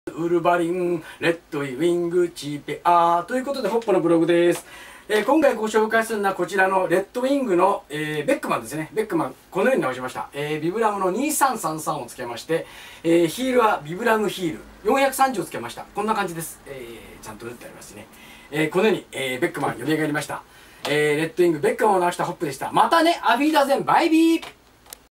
レッドウィング ベックマンのソール交換
ソールはビブラムソール２３３３
ヒールはビブラム４３０ヒールを取り付けました。